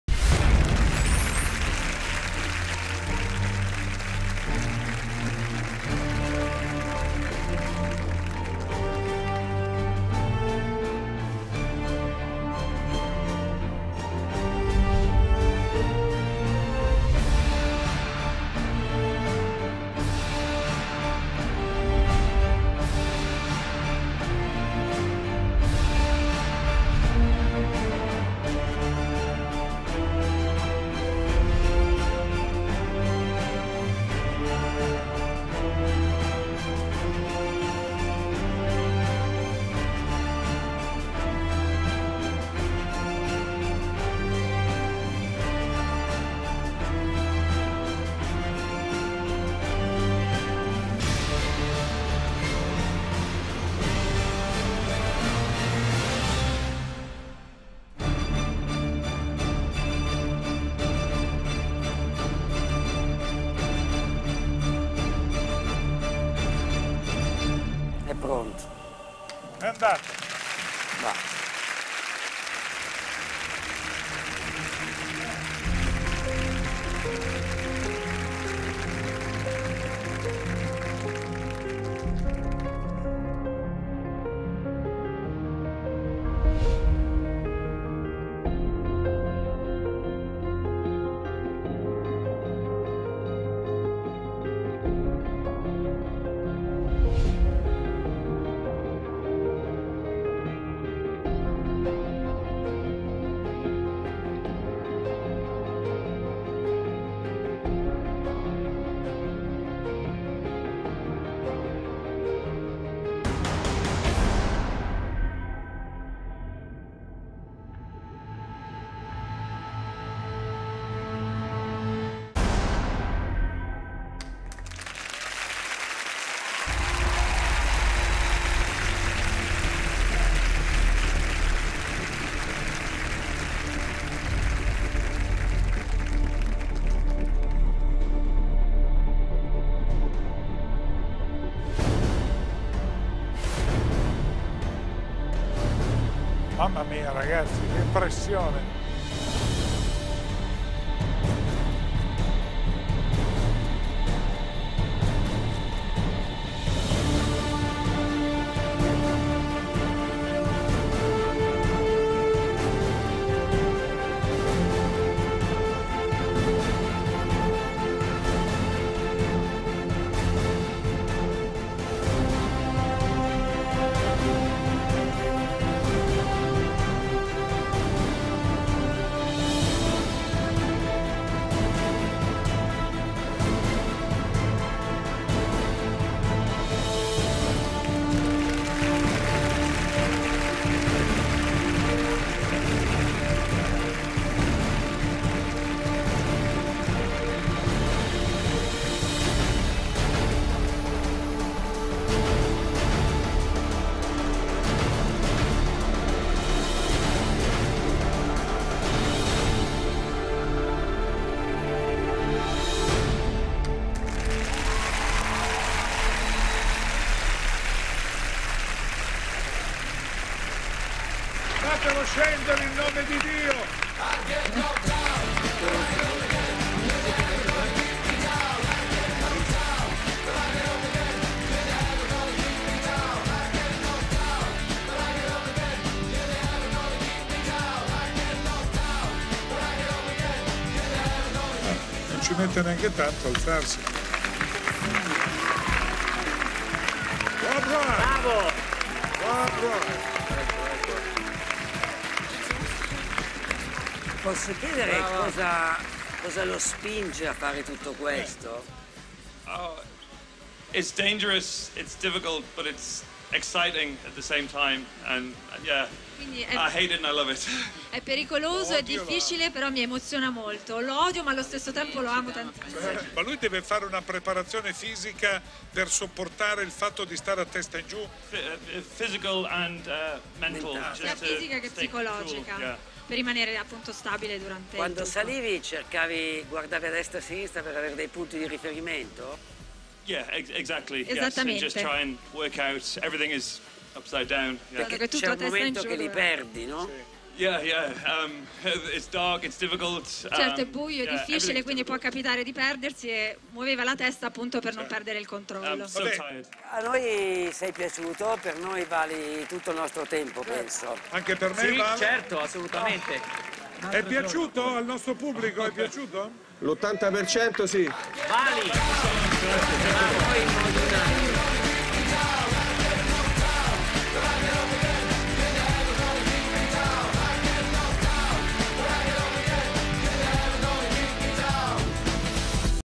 Rob Roy Collins upside down escape
Escape artist Rob Roy Collins performing an upside down strait jacket escape for TV.